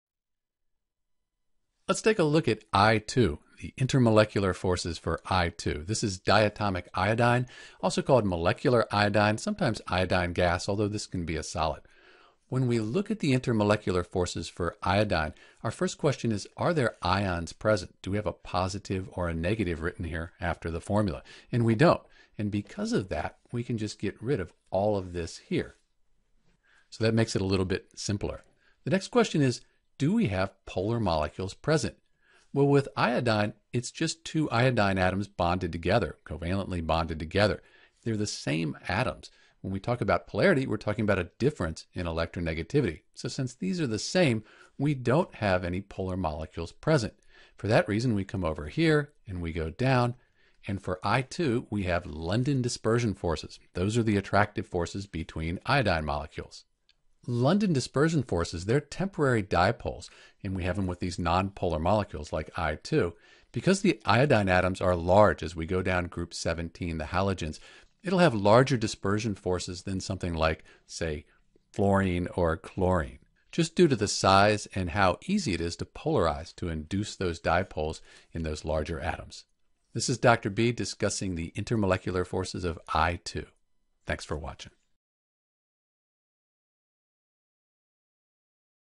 Intermolecular Forces for I2 (Diatomic Iodine)
In this video we’ll identify the intermolecular forces for I2 (diatomic Iodine / molecular Iodine).  Using a flowchart to guide us, we find that I2 only exhibits London Dispersion Forces since it is a non-polar molecule.

In determining the intermolecular forces present for I2 we follow these steps:

- Determine if there are ions present.  Since there is not + or – sign after the I2 we can say that it is not an ion.

- Next, based on its Lewis Structure, we determine if I2 is polar or non-polar Since there is no difference in electronegativity between the atoms I2 is non-polar.

- Because I2 is non-polar it will only exhibit London Dispersions Forces.